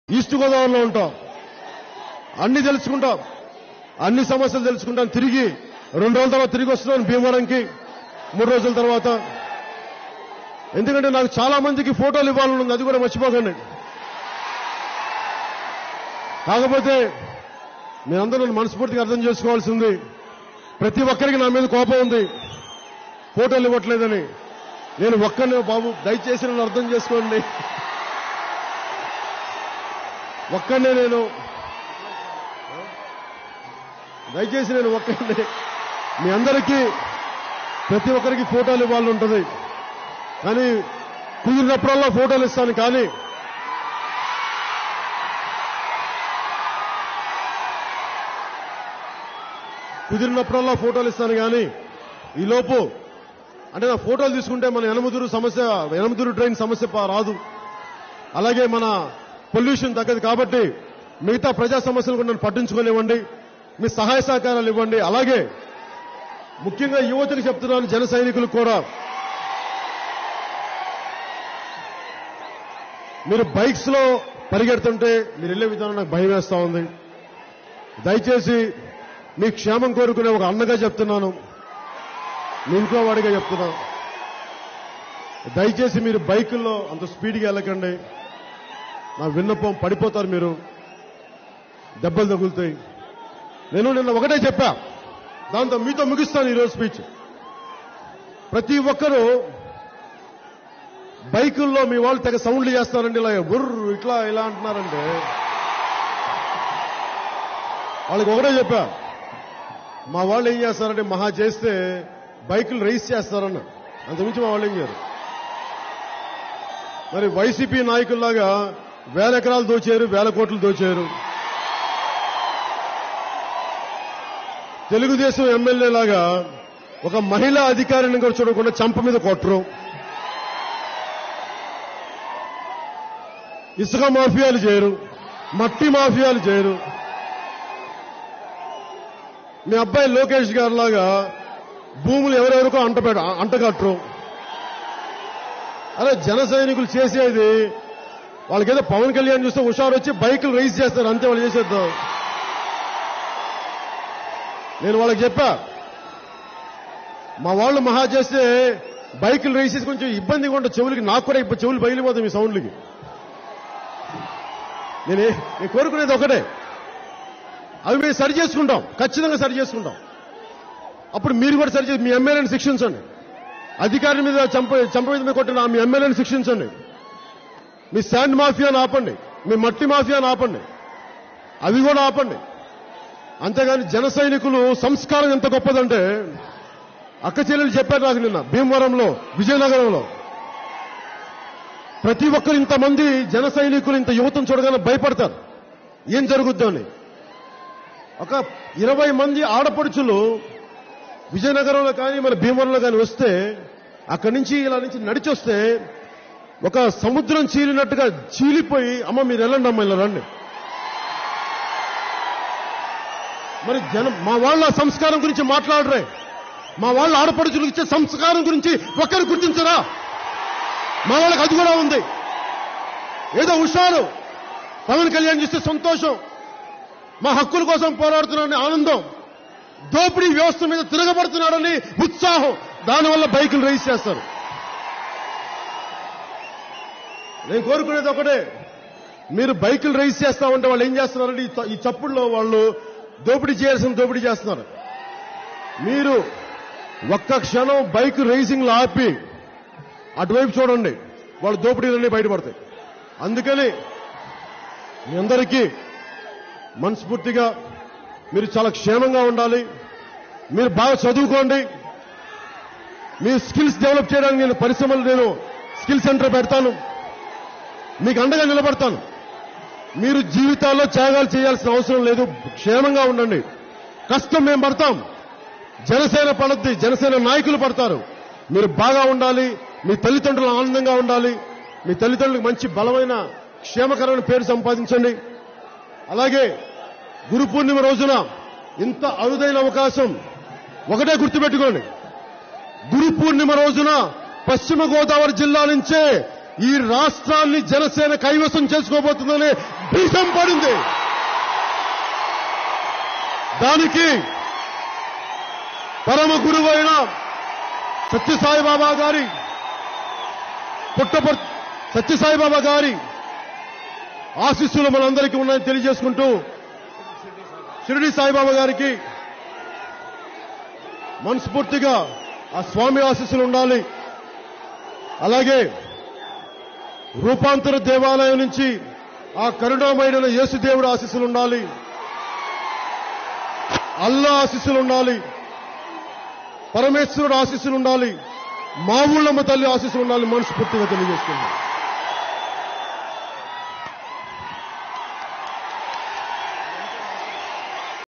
నారా లోకేష్ కు పవన్ పంచ్ | Pawan counter to Nara Lokesh | 99 TV
నారా లోకేష్ కు పవన్ పంచ్ janasena chief Pawan kalyan counter to tdp  Nara Lokesh

99%'s news is biased - bias not towards a political group but biased in favour of people.